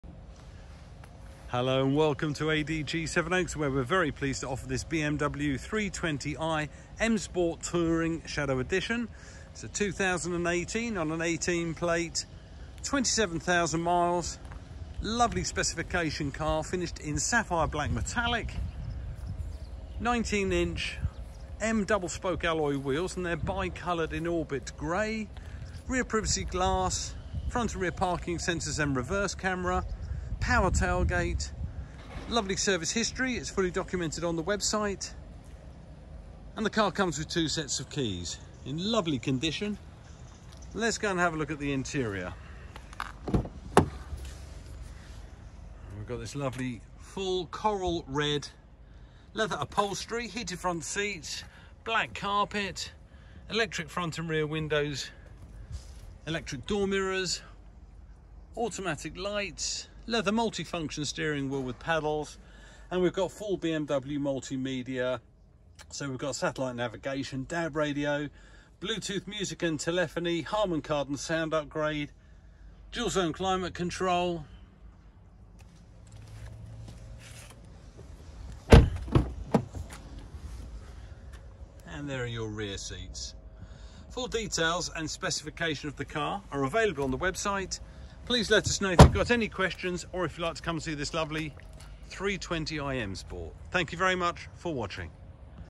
BMW 3 Series 2.0 320i M Sport Shadow Edition Touring Auto.
BMW 3 Series 2.0 320i M Sport Shadow Edition Touring Auto (s/s) in Sapphire Black Metallic. Great spec includes full Coral Red Leather Upholstery with Heated Front Seat and Black Carpets, BMW Multi Media with Satellite Navigation, Bluetooth Telephony and Driver Info Centre, Harman Kardon Surround Sound with Bluetooth Audio/DAB Digital Radio/USB Port, Reverse Camera with Front and Rear Parking Sensors, 19 inch M Double Spoke Style (704M) Alloy Wheels in Bi-Colour Orbit Grey, Cruise Control, Multi function Leather Steering Wheel, Rear Sun Protection Glass, supplied and serviced by John Clark BMW Aberdeen at 19727 miles in 03/20, MOT until 03/22, (we will provide a new MOT prior to sale), 2 x keys, superb throughout.